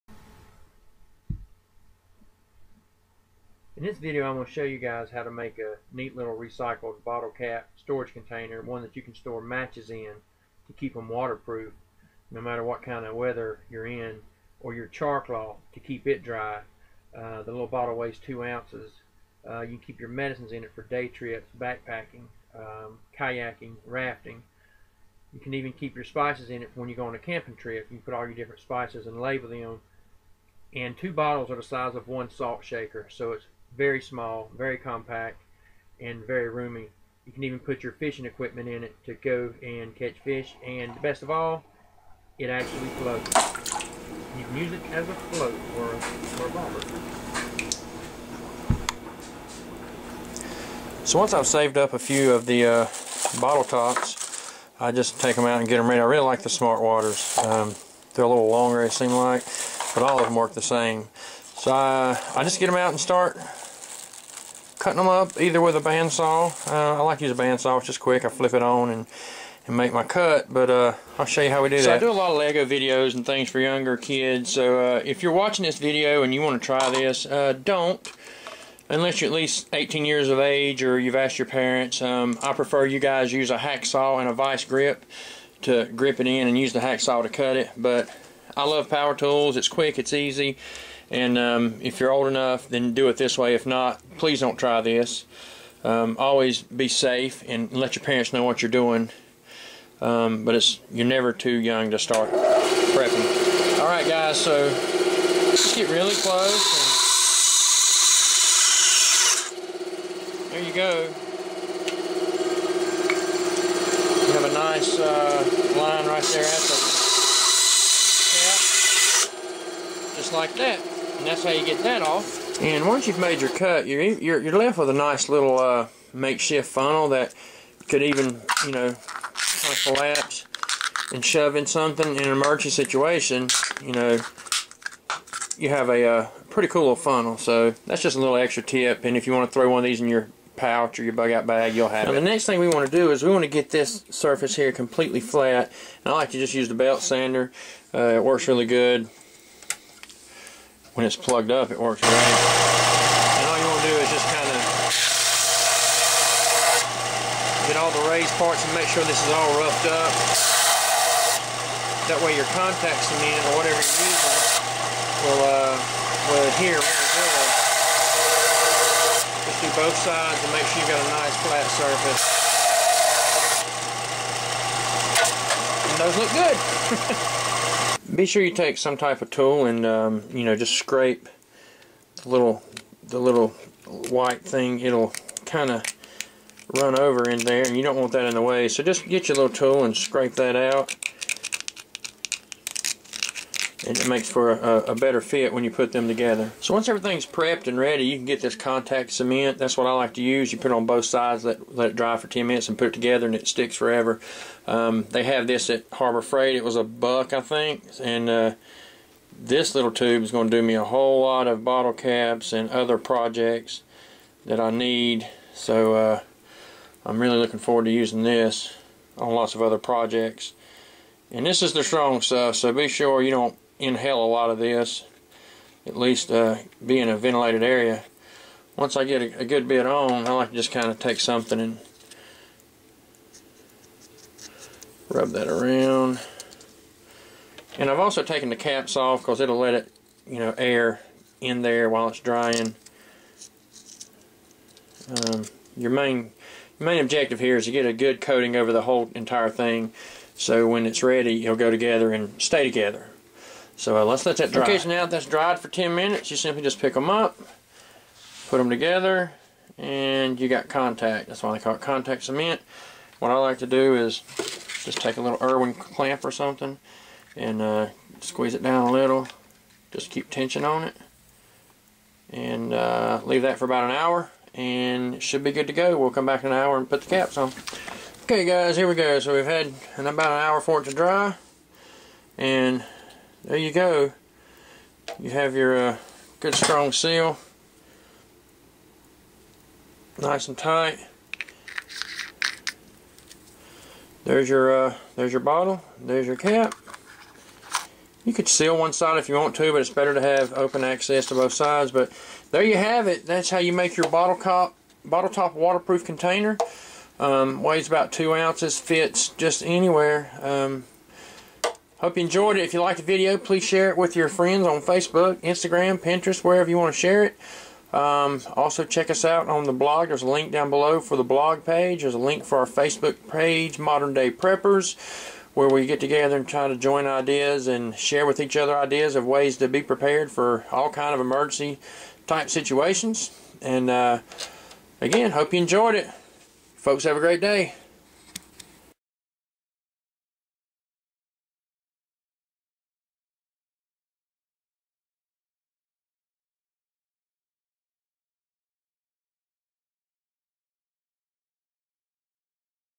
How To Make Waterproof Bottlecap Container
This is a great way to recycle those drink bottles and get all kinds of uses from food spices to emergency prepper/survival items!!

or